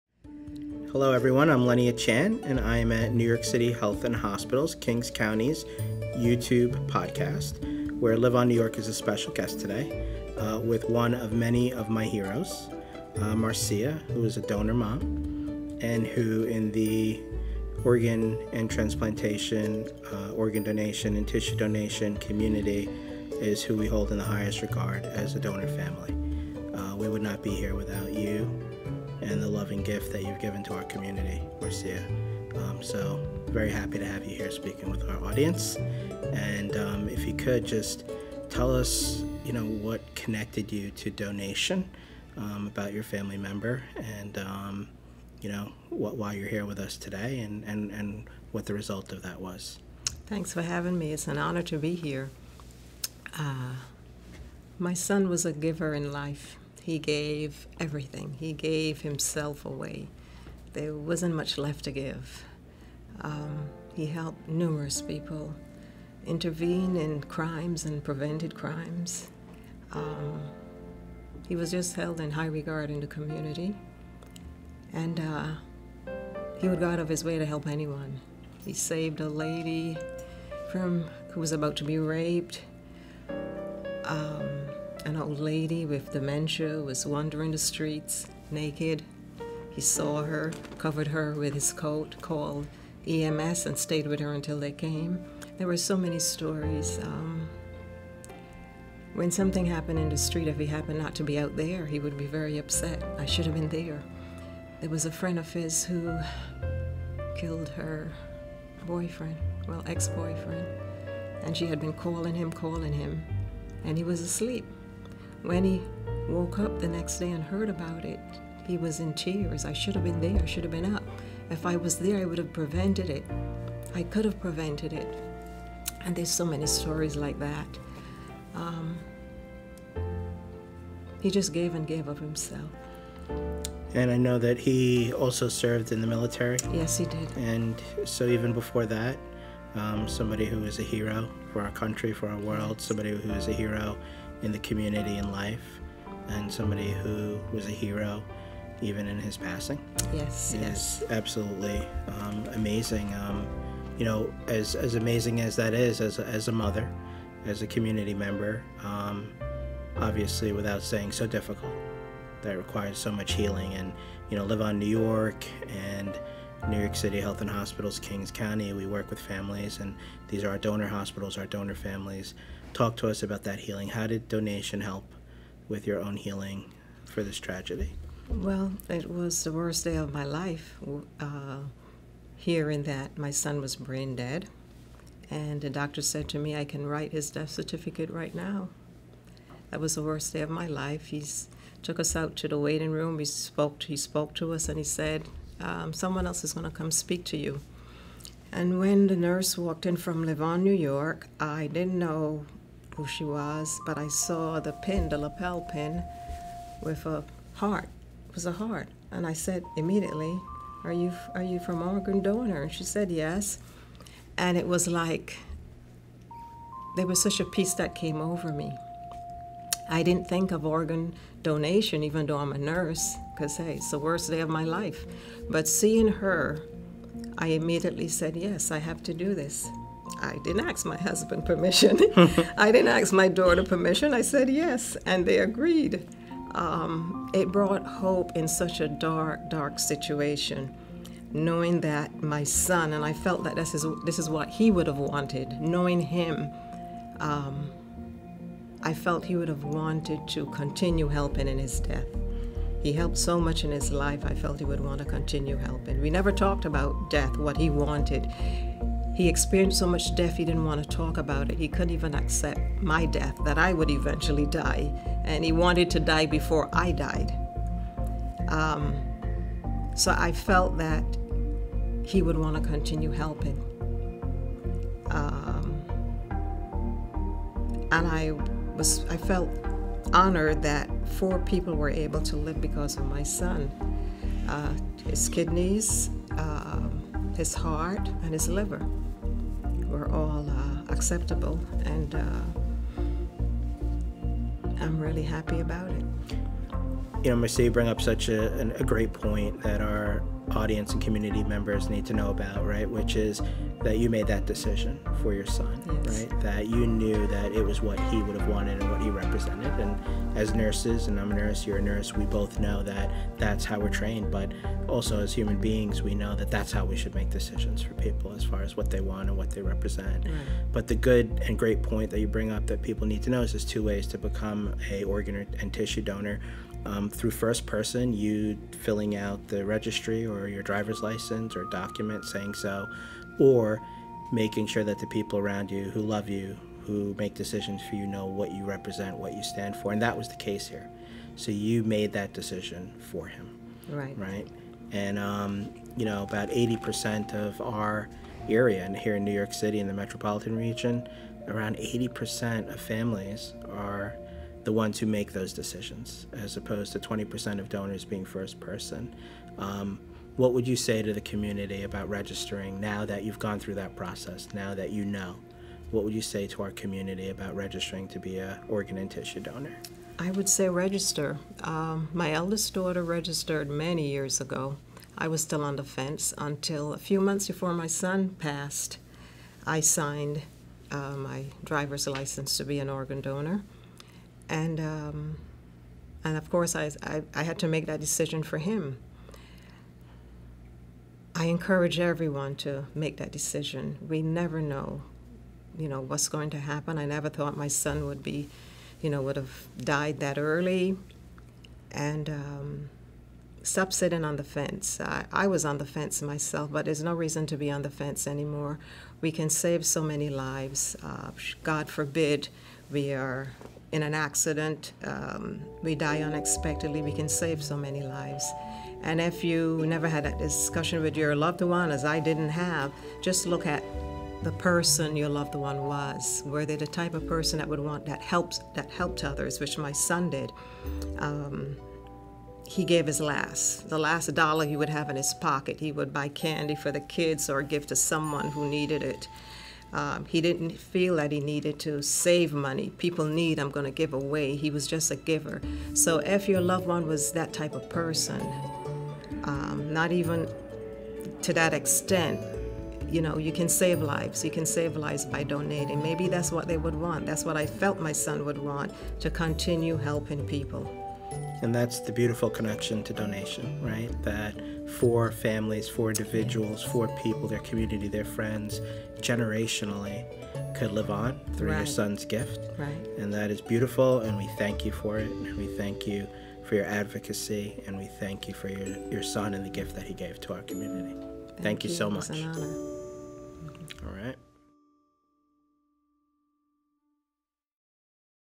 Podcast | LiveOnNY, Leonard Achan President & CEO, Ep 19 - Marcia Warner, Donor Mom & Volunteer
NYC Health + Hospitals/Kings County's YouTube Podcast Ep.19, features LiveOnNY President & CEO Leonard Achan, with Donor Mother and LiveOnNY Volunteer, Marcia Werner, sharing her story.